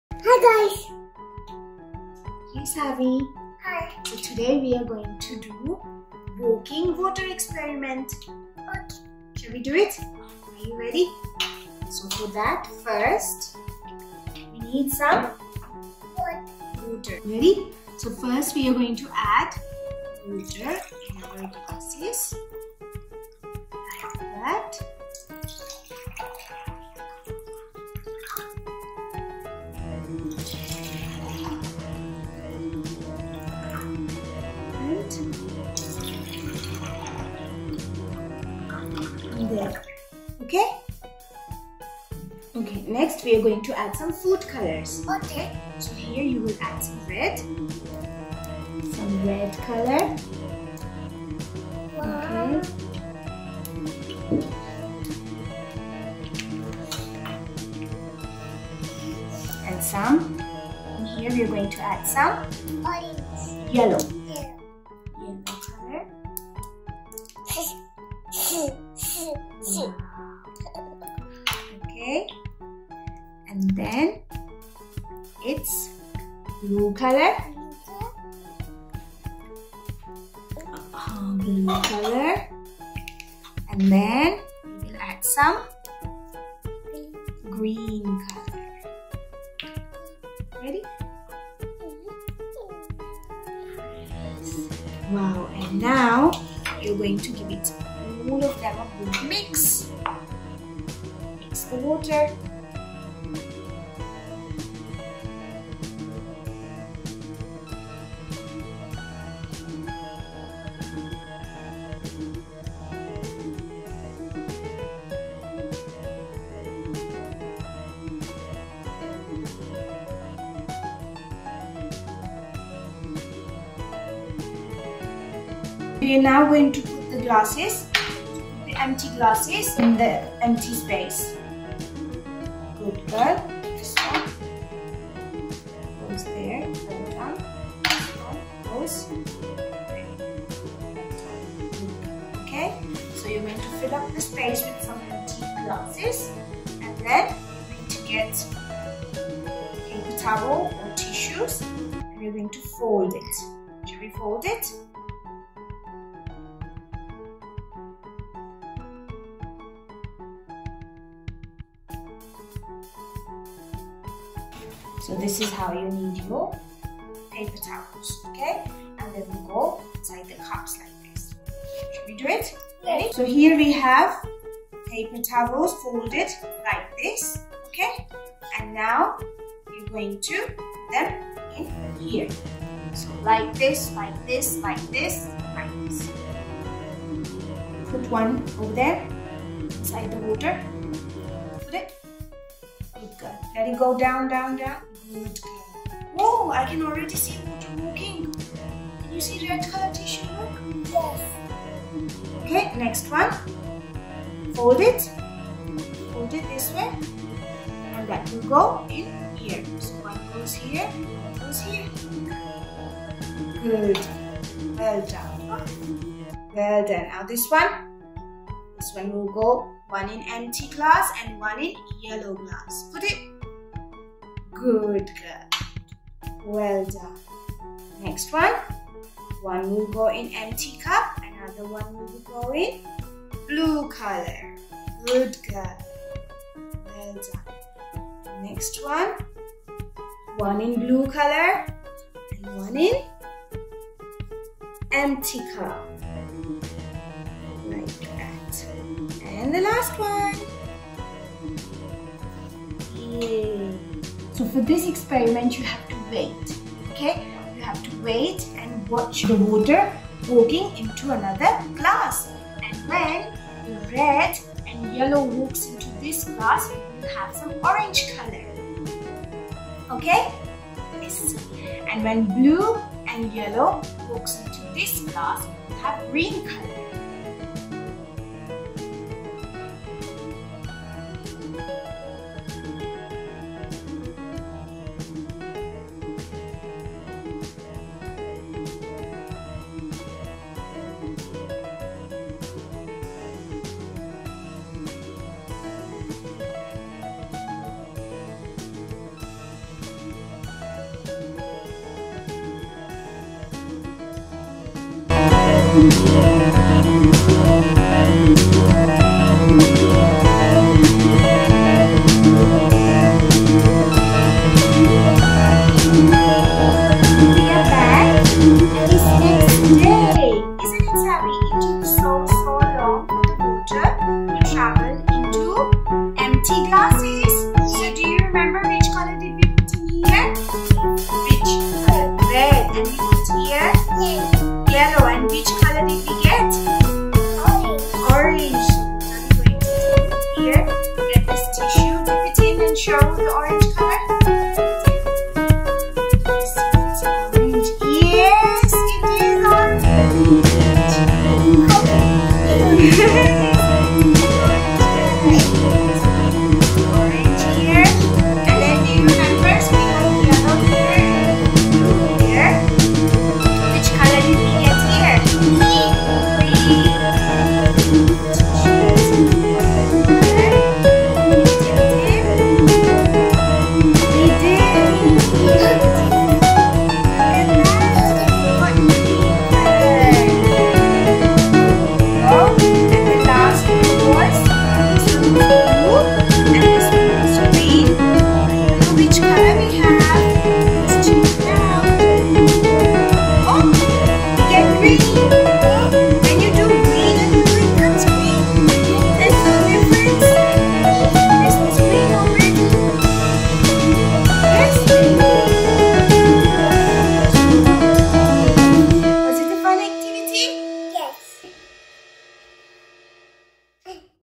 Walking Water Science Experiment for Kids | kids science experiments LEARNING COLORS
Saavi does walking water experiment using food colors and water